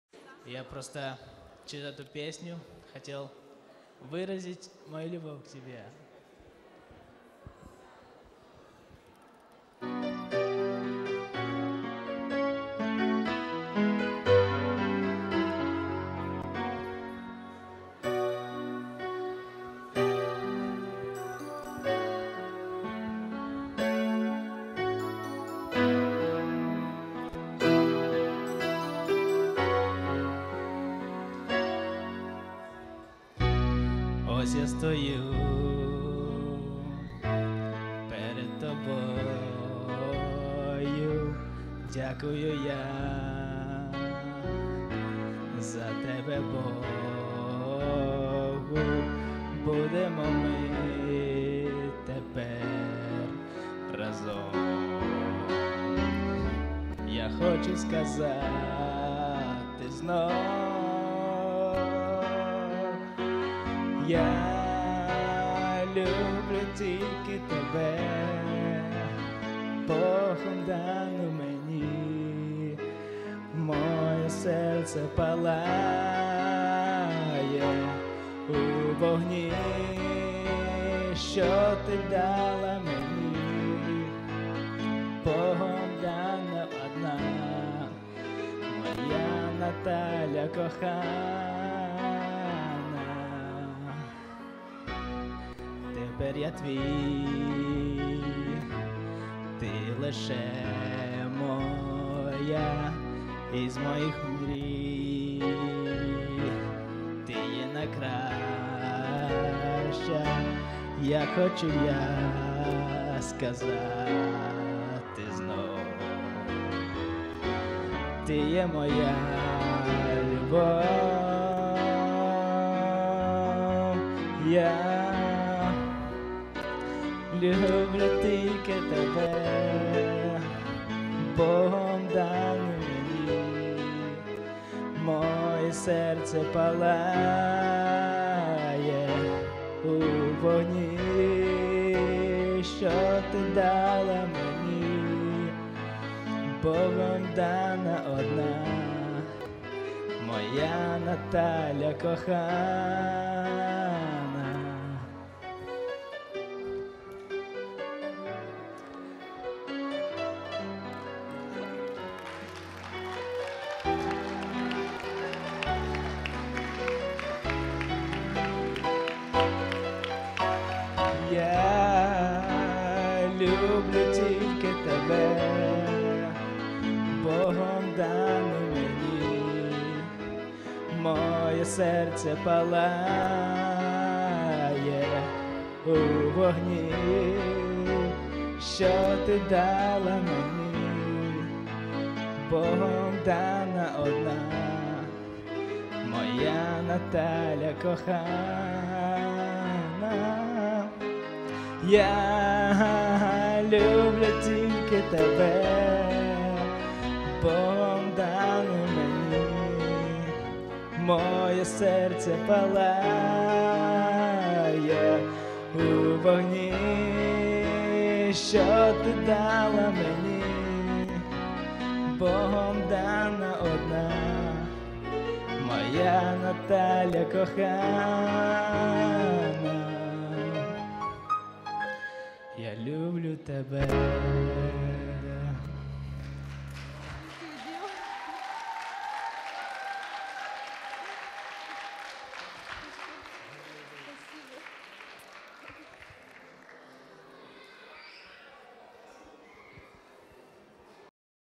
Husband sings to his wife on the wedding
Dima+Natasha Wedding

Stabiluss Studios
Wedding Videographers in Spartanburg, SC